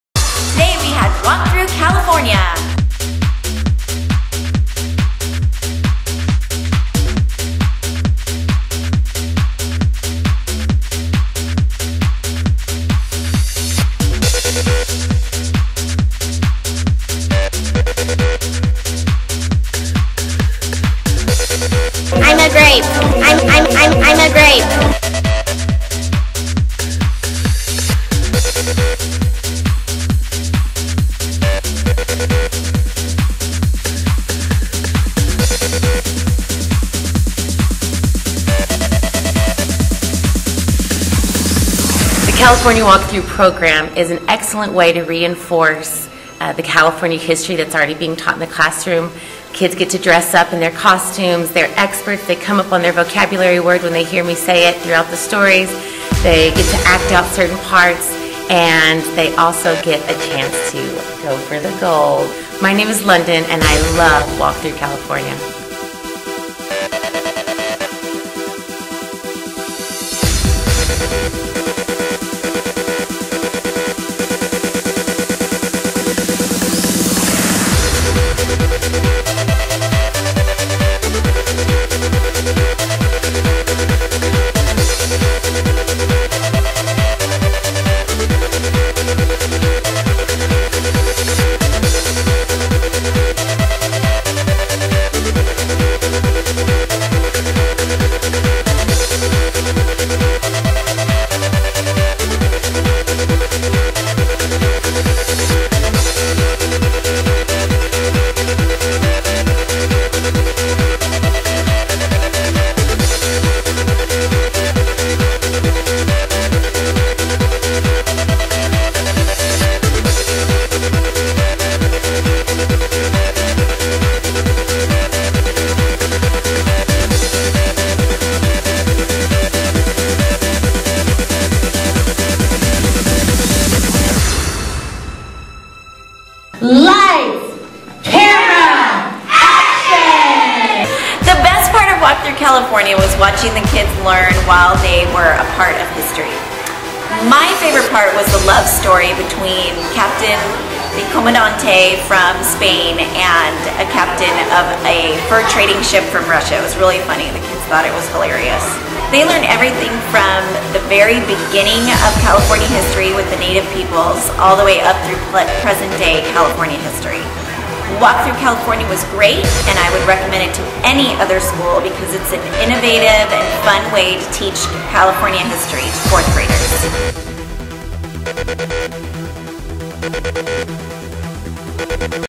Poinsettia Elementary Walk Thru California 2014
Poinsettia Elem. Overview of the walk thru California Assembly.